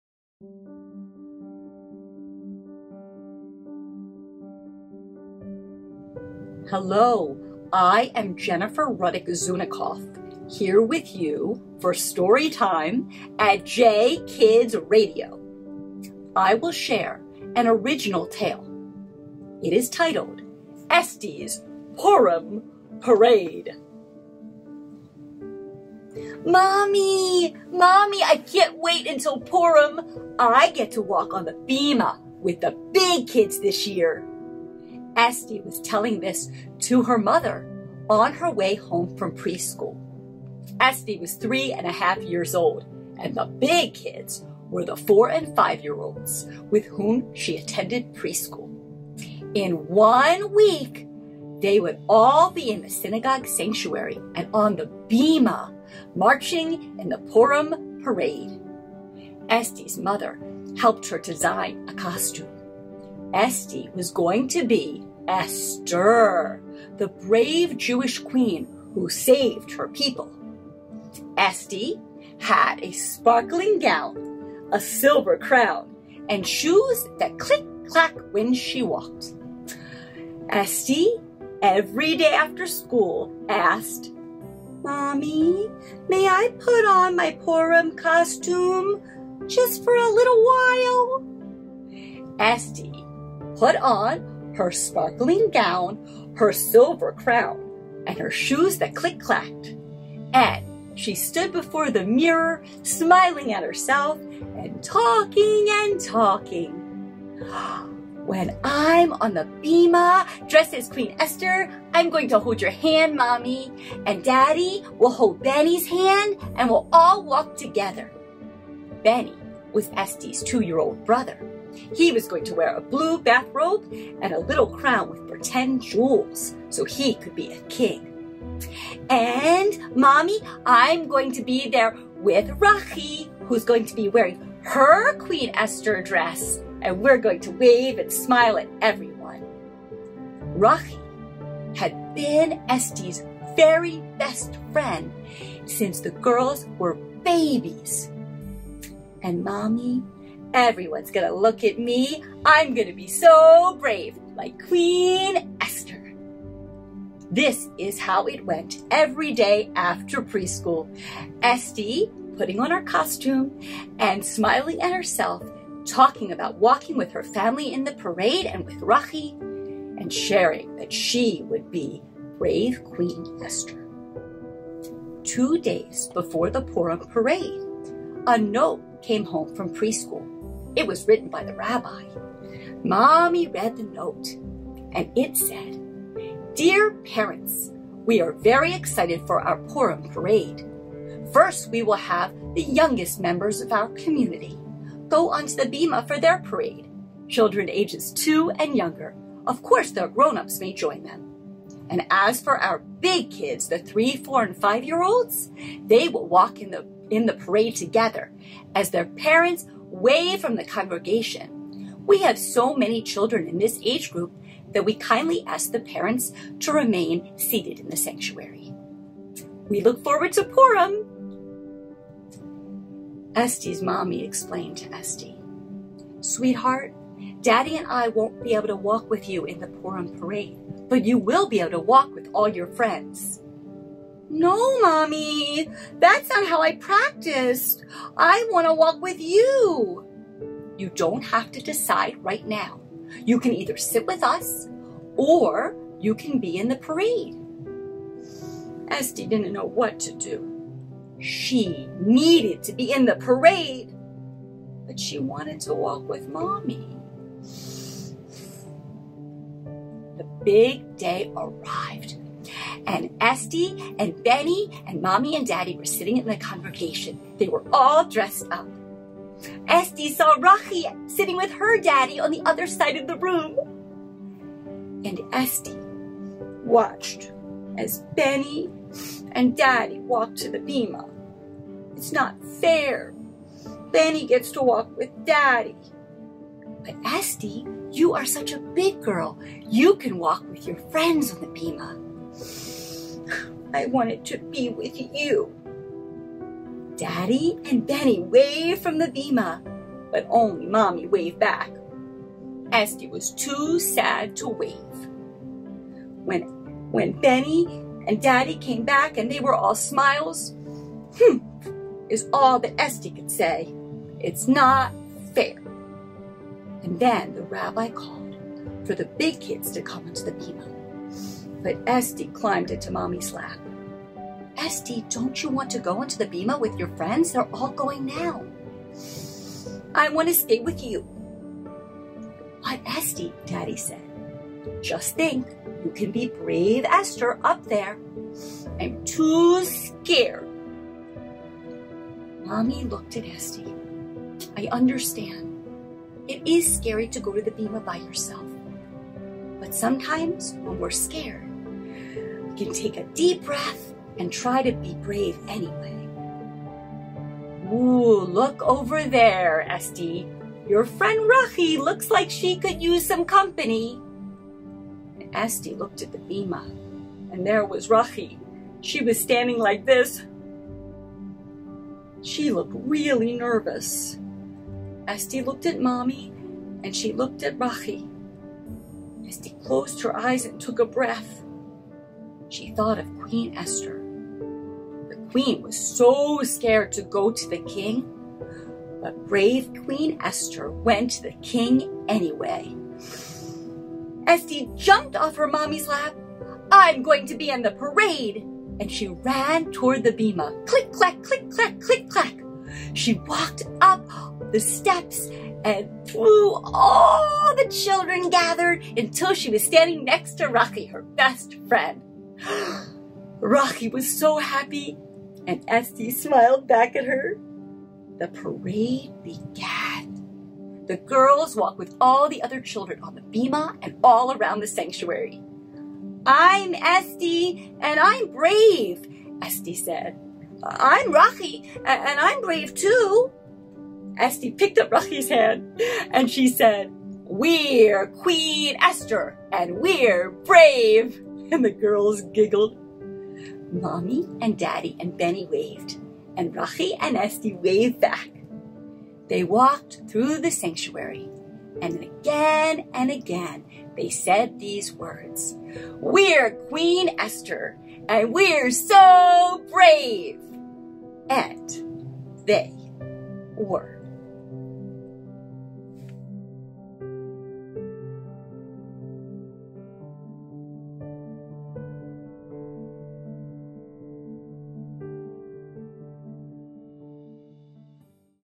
JKids Radio - StoryTime with Jennifer Rudick Zunikoff - Esti's Purim Parade
Original Purim story by Jennifer Rudick Zunikoff